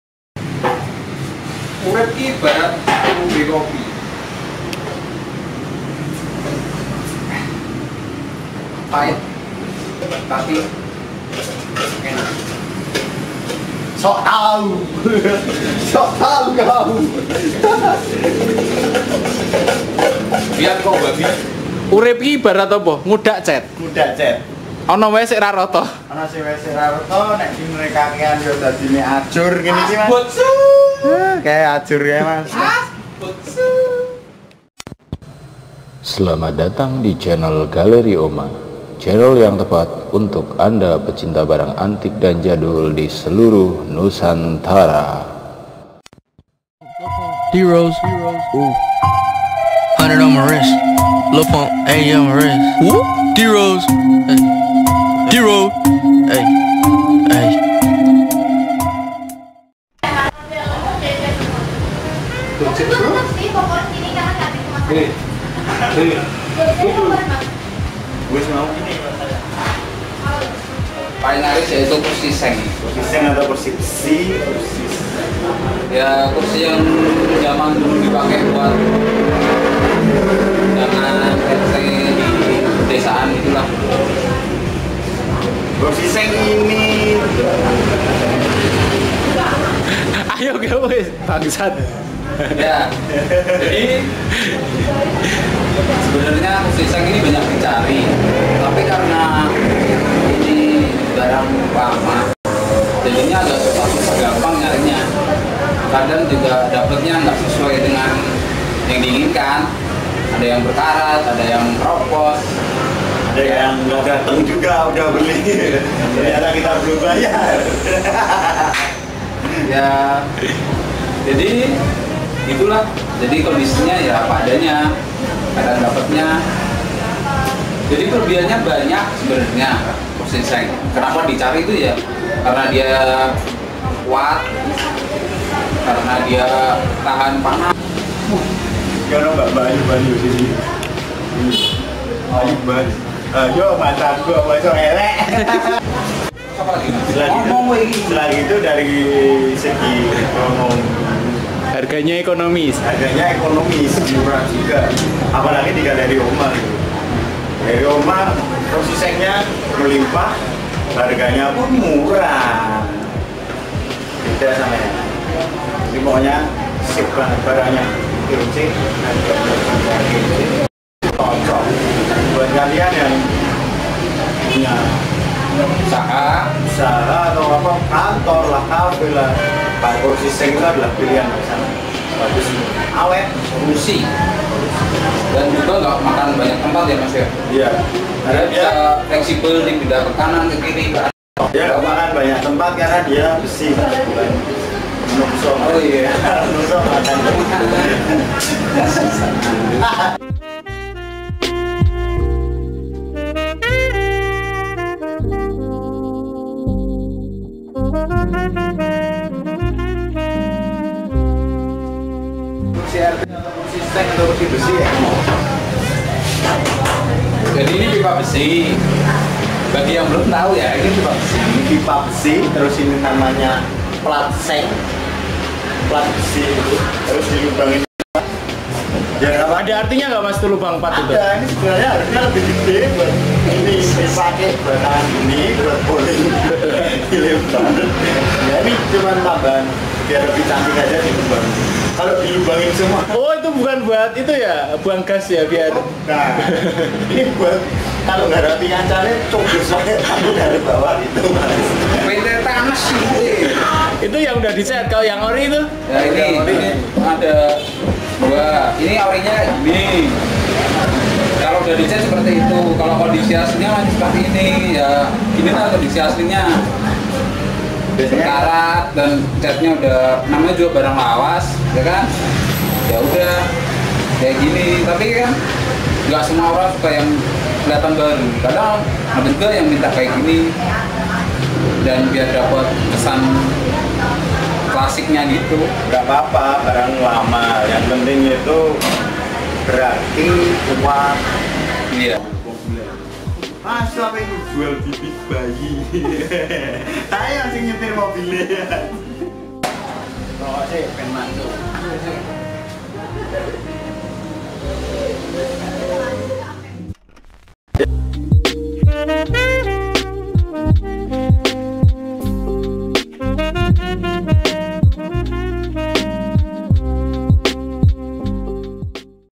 CERITA KEHIDUPAN SELES KURSI
Mas mbarep dan mas cis sedang menceritakan kursi terlaris di galeri oma. Kursi besi yg biasa untuk acara hajatan di kampung. Kursi ini banyak dicari untuk kursi makan dicafe atau ngopi di warung kopi. Selain murah karna kuat juga. Semoga menginspirasi 😁